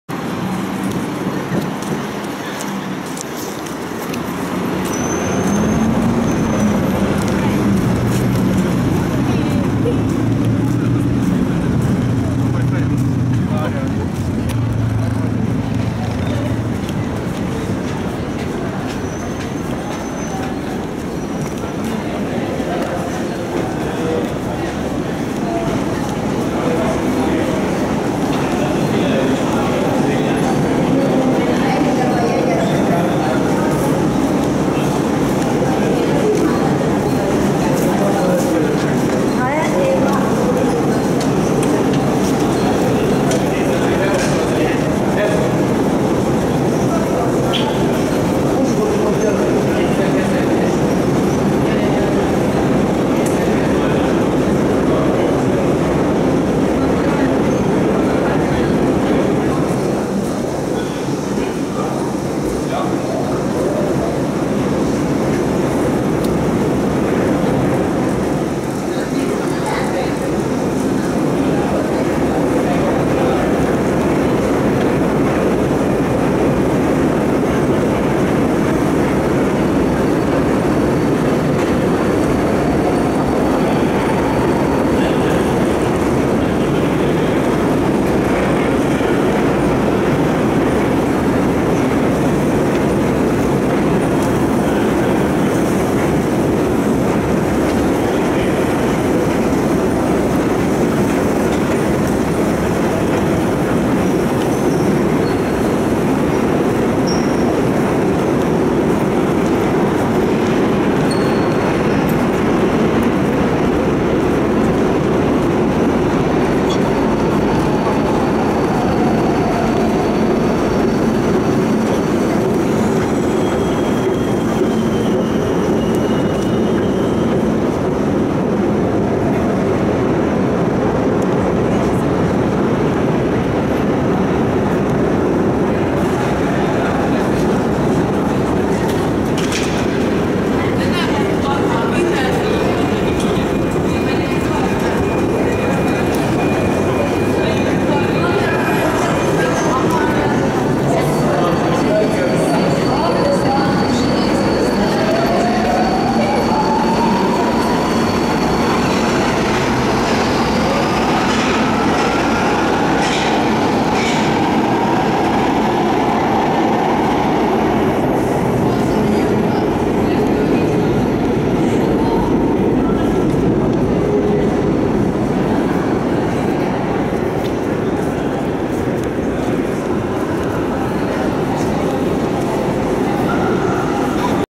M4 Metro Szent Gellért tér
Going down the new M4 Metro (4-es Metró) at Szent Gellért tér in Budapest, Hungary.

Technical Specifications:
Camera: Sony HDR-CX410VE Handycam (2013 model)
Negative Format: AVCHD 1080p/25

Filming Dates:
April 2014